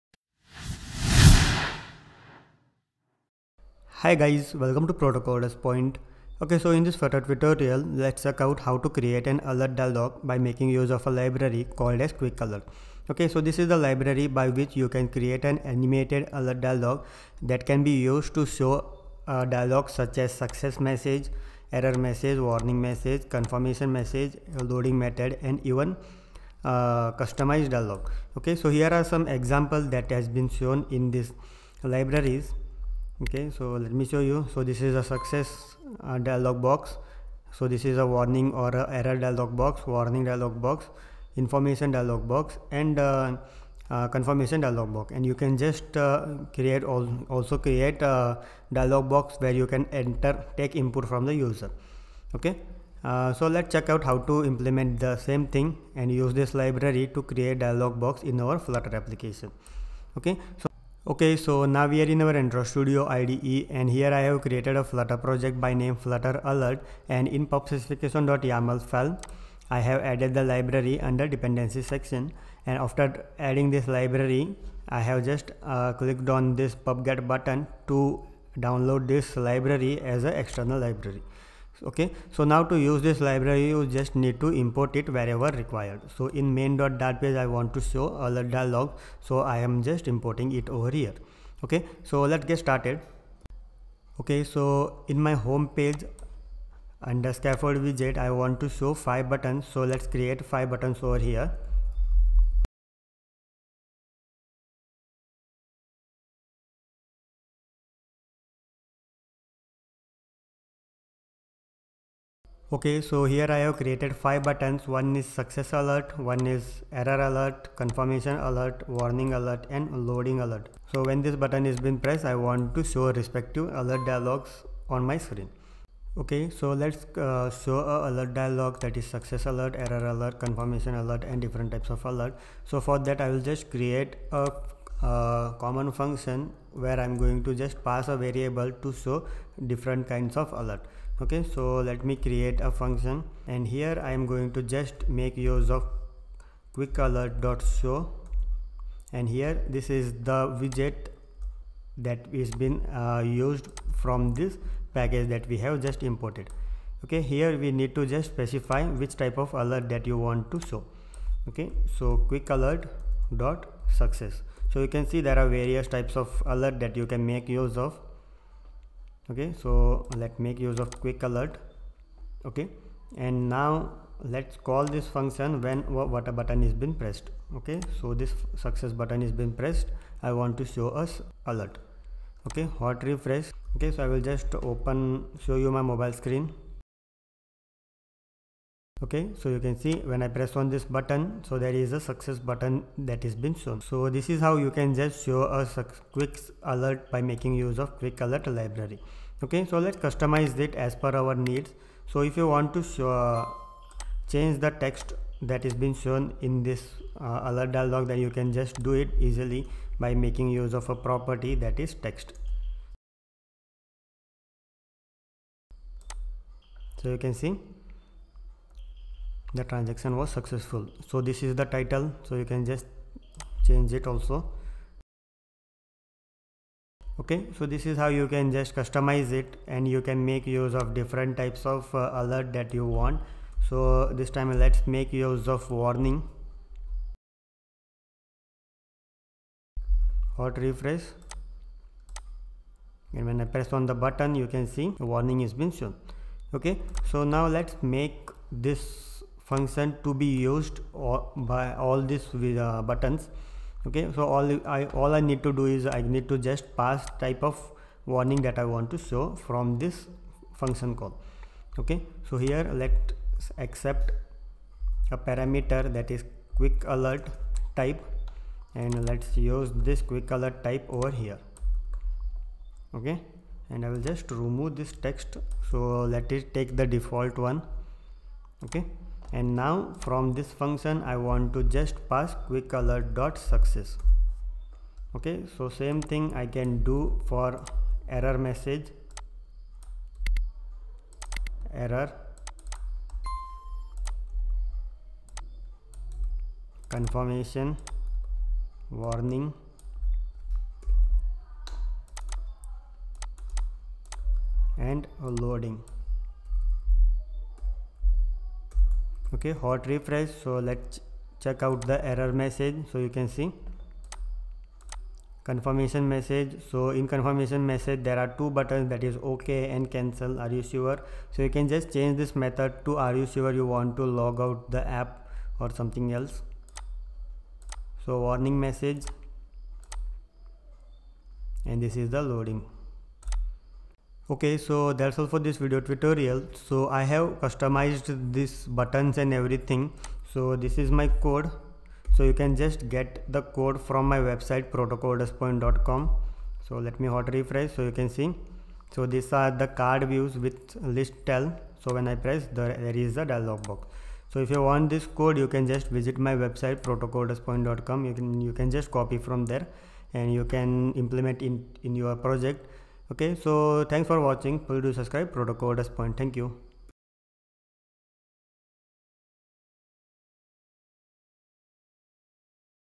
Flutter QuickAlert: Easiest Way to Show Alert Dialog in Flutter App
Flutter quickalert

In flutter, If you are willing to show a Alert dialog then quickalert package is the best library to use. It’s an animated alert dialog using which we can show dialop popup alert like success alert, error alert, warning alert, confirm alert dialog box with just few lines of code.